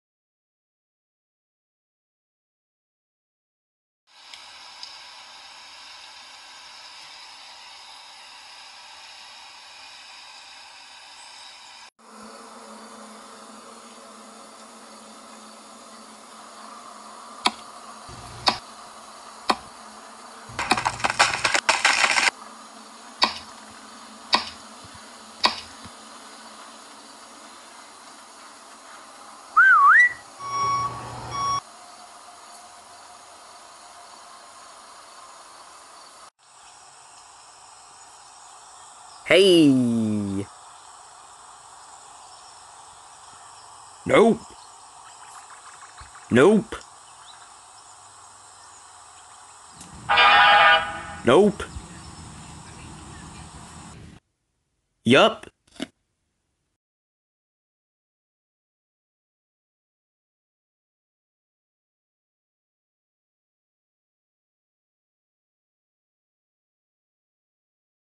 🌺 Flower Pot Girl: The Movie
Following a few days since the trailer, we see the incredible and also NOT true story. The Lumberjack spends his days chopping down trees with his assistant. However, one day everything will change.